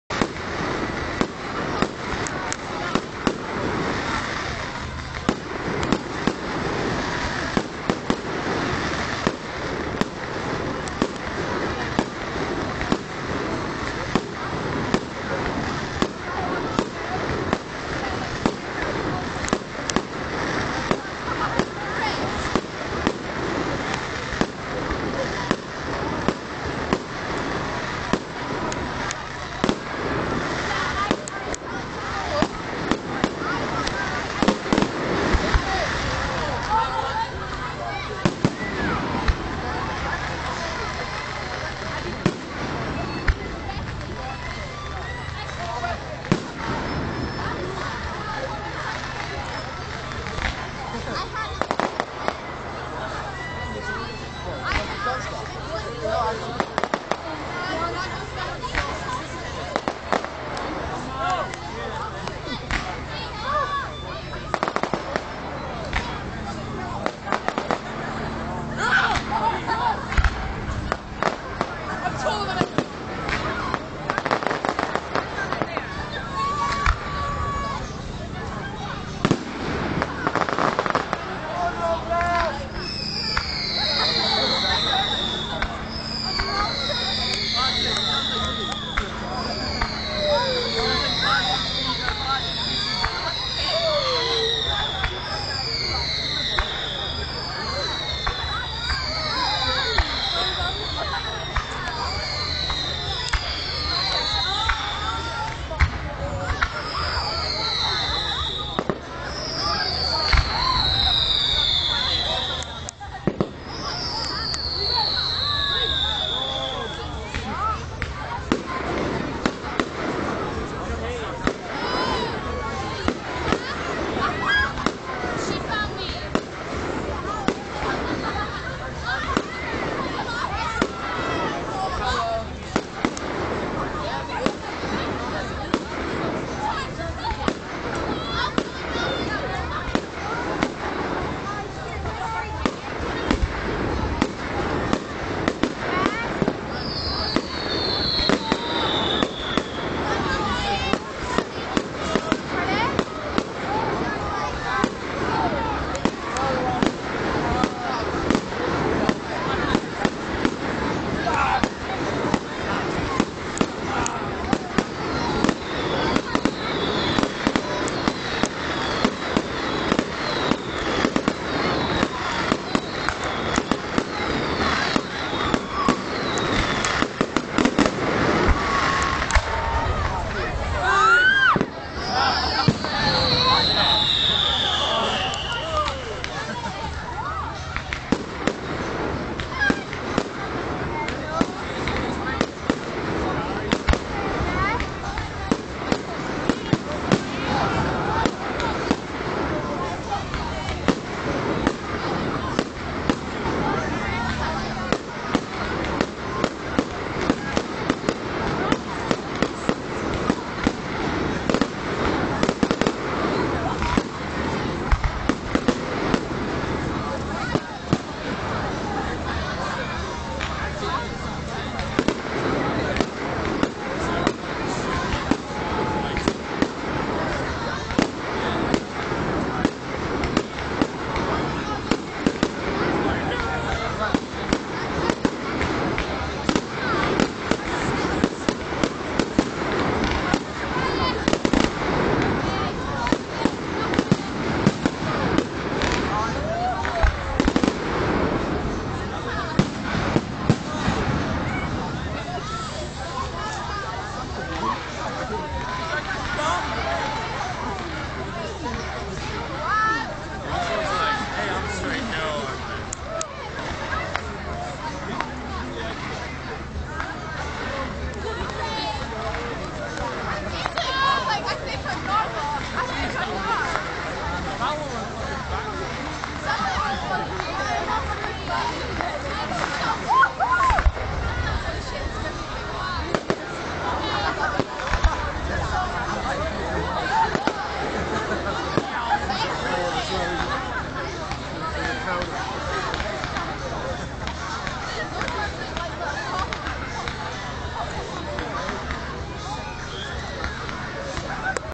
Part 2 of Forres 2010 Fireworks 06/11/2010
small clip of the fireworks at Forres the night after bonfire night